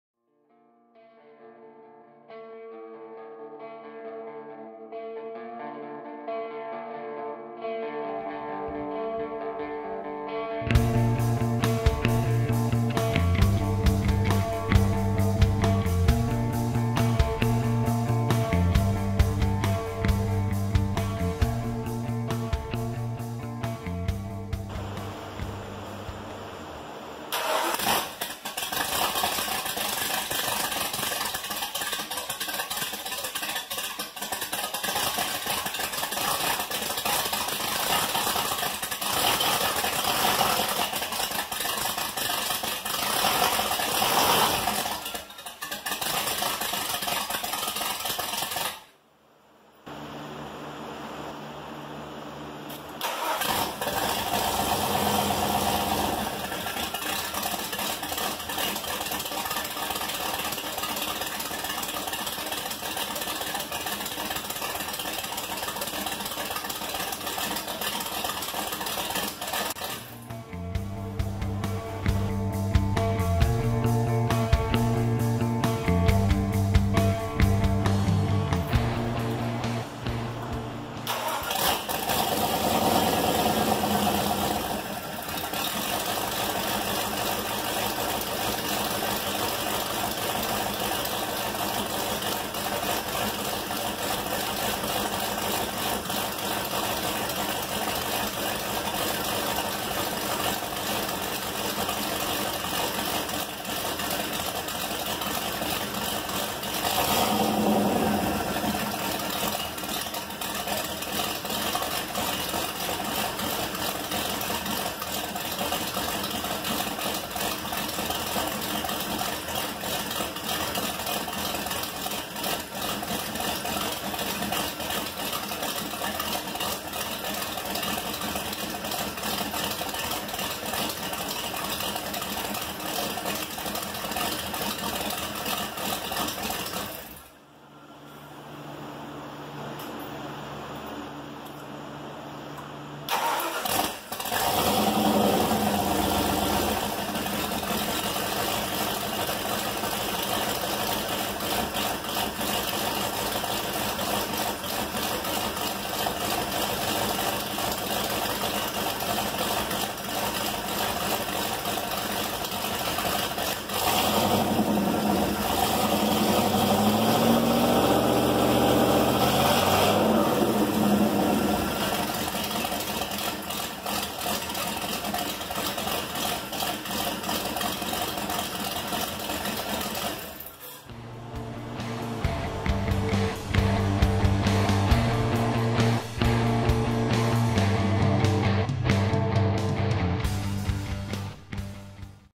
THUNDERBIRD SC - PROJECT GFP - FIRST START UP!
After a lot of work, sweat, and several setbacks it was time to start this beast.  To make sure the engine was functioning correctly, the turbo wasn't installed for simplicity sake.  Enjoy!

PROJECT GFP (Glutton For Punishment)

Sorry for the re-upload.  I had to fix something with the original video.